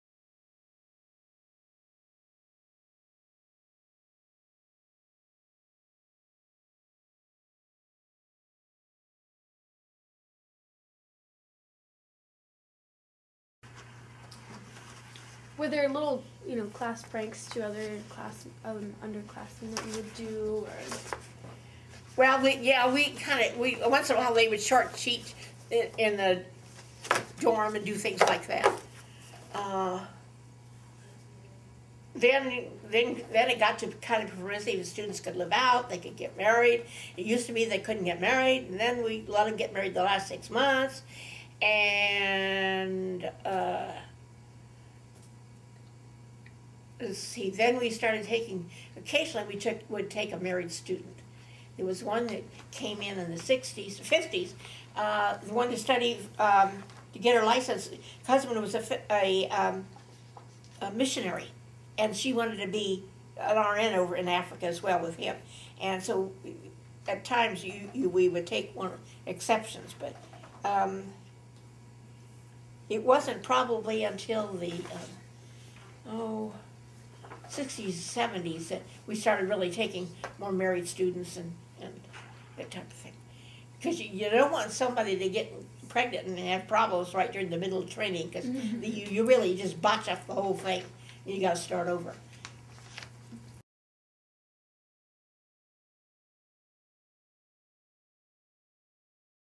Barbara Jury on regulations and requirements for the students of CHSN
Barbara Jury, nurse and former director of Nursing for the California Hospital, describes the evolution of CHSN's regulation around admitting married students to the nursing school. The interview is part of the CHSN Interview Project and Dr. Sharon Sekhon conducted the interview.